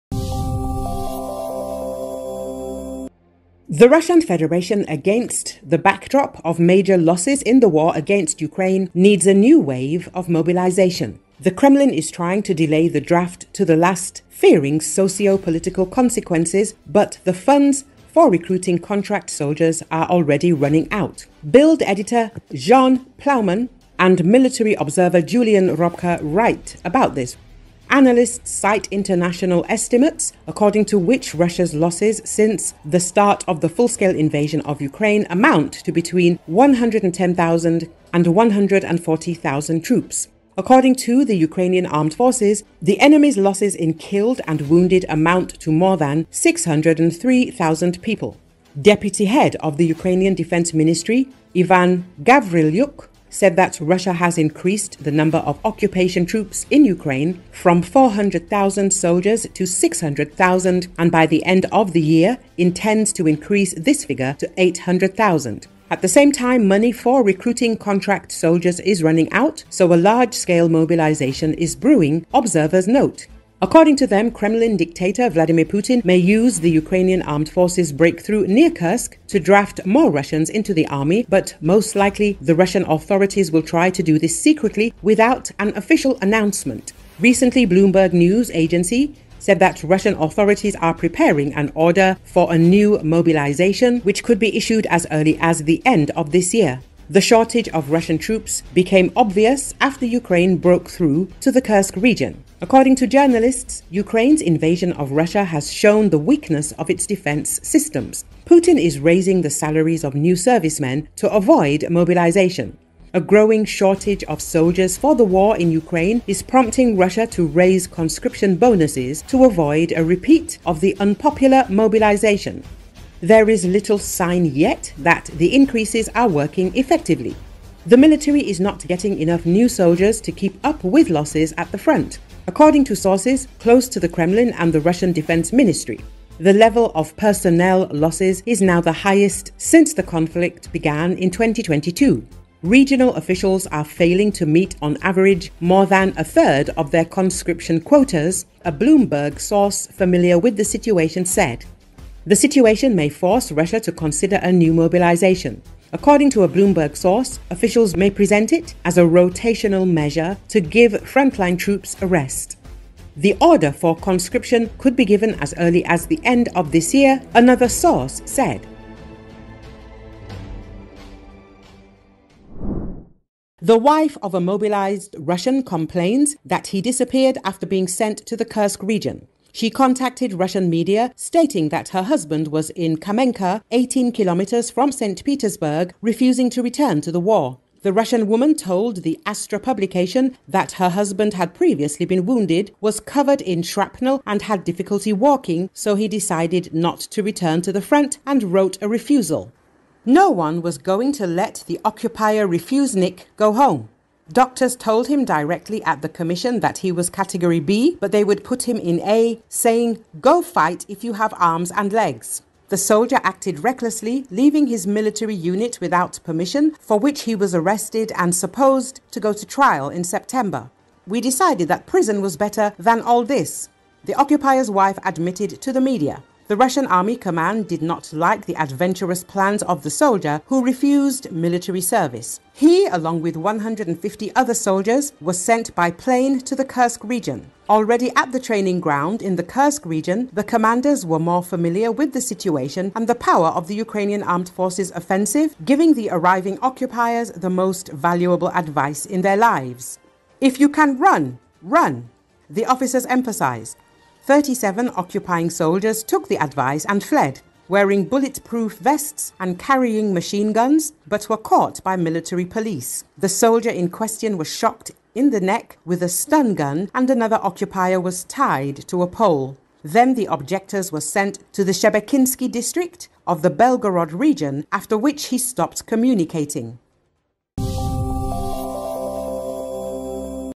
Russia is running out of money to recruit contract soldiers, large-scale mobilization is brewing
The Russian Federation, against the backdrop of major losses in the war against Ukraine, needs a new wave of mobilization. The Kremlin is trying to delay the draft to the last, fearing socio-political consequences, but the funds for recruiting contract soldiers are already running out.  Bild editor Jeanne Plaumann and military observer Julian Repke write about this . Analysts cite international estimates, according to which Russia's losses since the start of the full-scale invasion of Ukraine amount to between 110,000 and 140,000 troops. According to the Ukrainian Armed Forces, the enemy's losses in killed and wounded amount to more than 603,000 people.  Deputy head of the Ukrainian Defense Ministry Ivan Gavrilyuk said that Russia has increased the number of occupation troops in Ukraine from 400 thousand soldiers to 600 thousand, and by the end of the year intends to increase this figure to 800 thousand.  At the same time, money for recruiting contract soldiers is running out, so a large-scale mobilization is brewing, observers note. According to them, Kremlin dictator Vladimir Putin may use the Ukrainian Armed Forces' breakthrough near Kursk to draft more Russians into the army, but most likely, the Russian authorities will try to do this secretly, without an official announcement.
Recently, Bloomberg news agency said that Russian authorities are preparing an order for a new mobilization, which could be issued as early as the end of this year. The shortage of Russian troops became obvious after Ukraine broke through to the Kursk region. According to journalists, Ukraine's invasion of Russia has shown the weakness of its defense systems . Putin is raising the salaries of new servicemen to avoid mobilization. A growing shortage of soldiers for the war in Ukraine is prompting Russia to raise conscription bonuses to avoid a repeat of the unpopular mobilization. There is little sign yet that [the increases] are working effectively. The military is not getting enough new soldiers to keep up with losses at the front,  according to sources close to the Kremlin and the Russian Defense Ministry. The level of personnel losses is now the highest since the conflict began in 2022. Regional officials are failing to meet on average more than a third of their conscription quotas, a Bloomberg source familiar with the situation said. The situation may force Russia to consider a new mobilization. According to a Bloomberg source, officials may present it as a rotational measure to give frontline troops a rest. The order for conscription could be given as early as the end of this year, another source said.